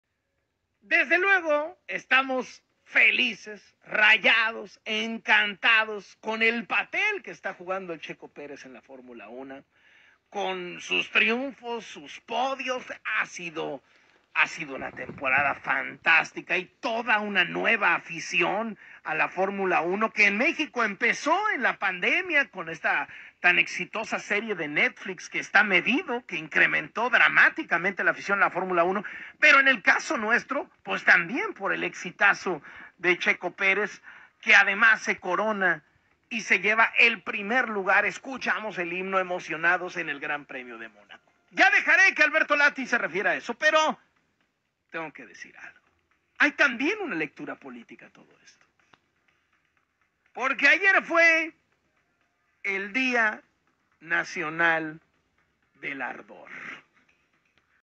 ¡EXCELENTE NOTICIA! LORET DE LATINUS MUY CONTENTO CON EL TRIUNFO DE CHECO PÉREZ! LE DUELE A ANLO!
Loret de Mola festejó en grande el triunfo de Checo Pérez, algo que no gustó en Palacio Nacional